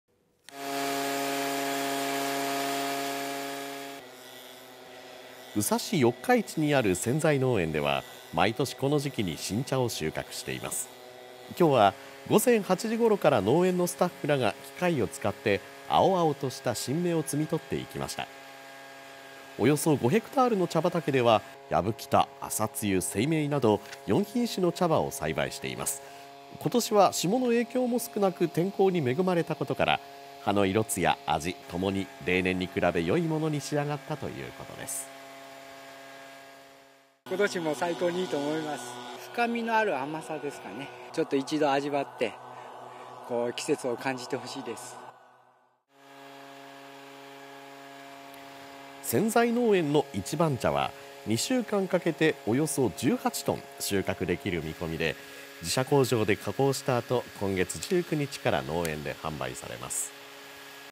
早くも新茶の茶摘みが始まる 5月1日の「八十八夜」を前に
5月１日の八十八夜を前に、大分県宇佐市の農園では早くも新茶の収穫作業が始まりました。